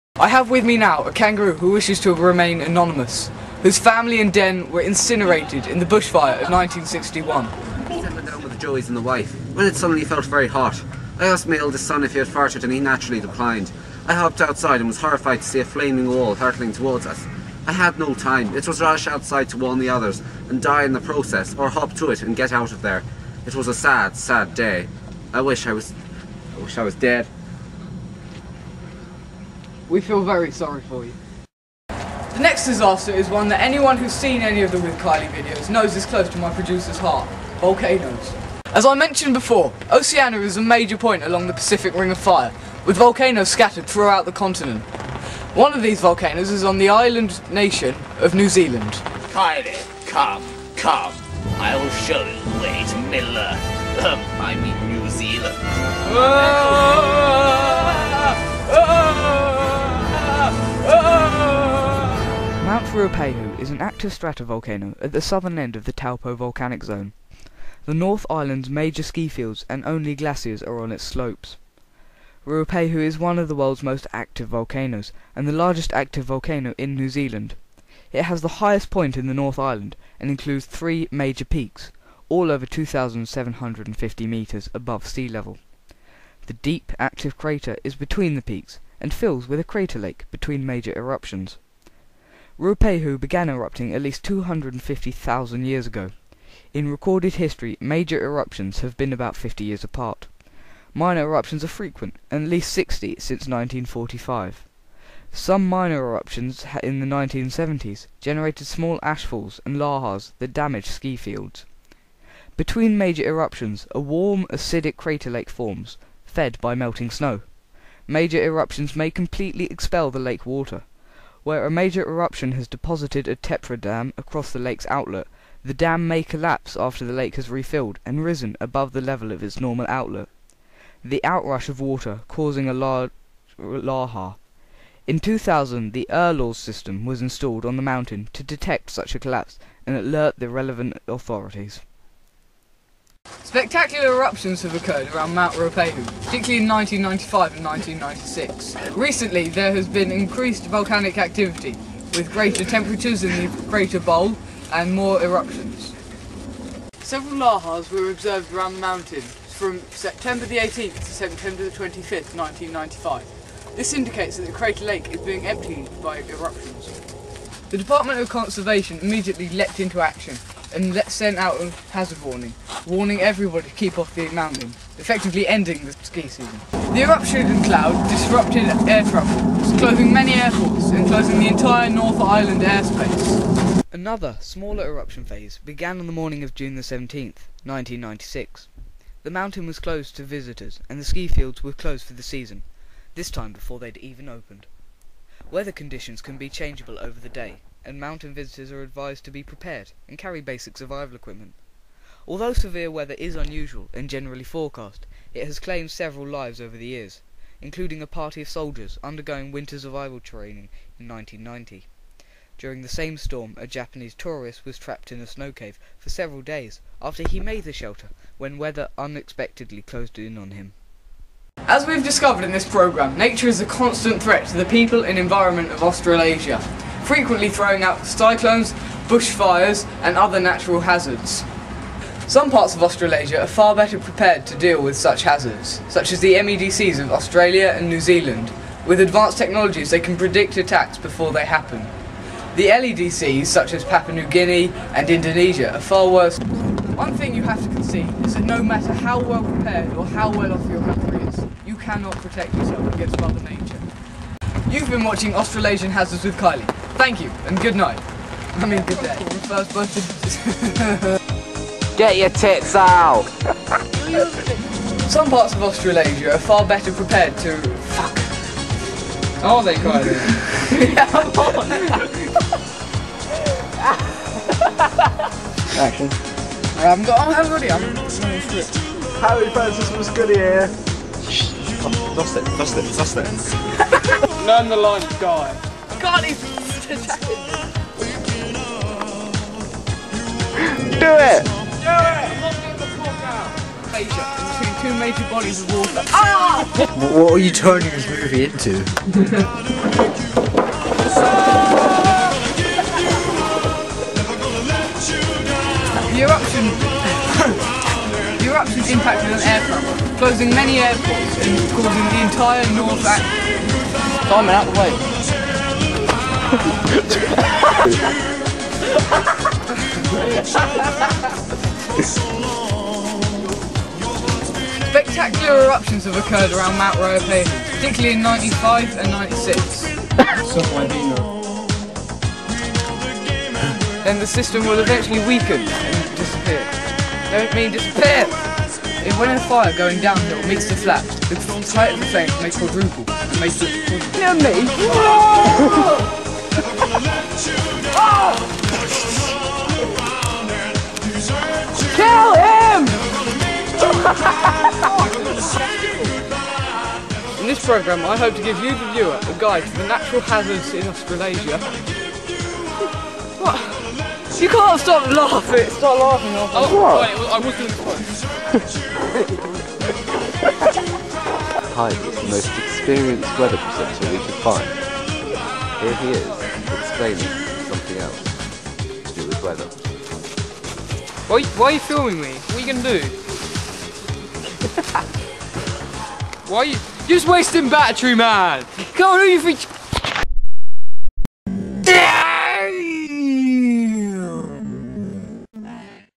Australasian Hazards With Kyley (Part 2)
The latest (3rd) in the series of with Kyley documentaries. This informative video explains the hazards present to those living in the continent of australasia. Filmed on location in Burnham Beeches by Burnham Grammar sixth form Geography students.
(Part 2) WITH OUTTAKES